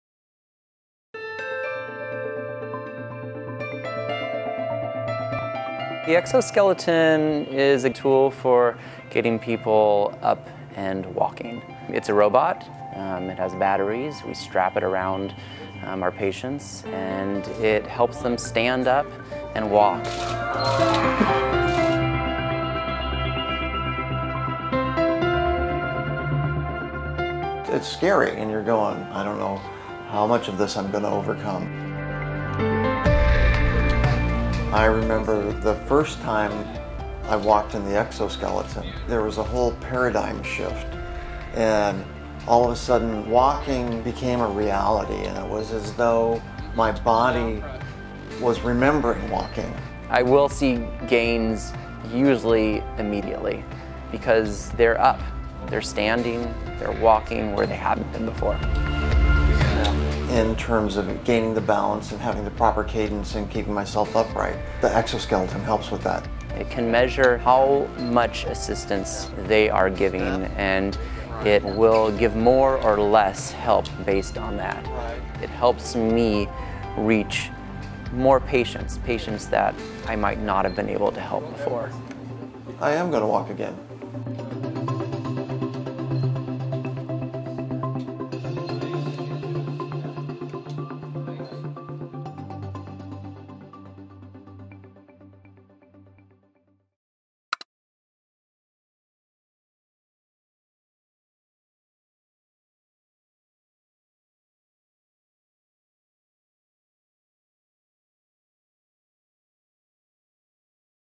Robotic Exoskeleton Helps People With Disabilities Walk Again
The Ekso Skeleton is a cutting-edge device that helps patients suffering from spinal injury and stroke stand and walk. Thanks to funding from the Cottage Rehabilitation Hospital Foundation, CRH in Santa Barbara is the only facility with the device between Los Angeles and the Bay Area.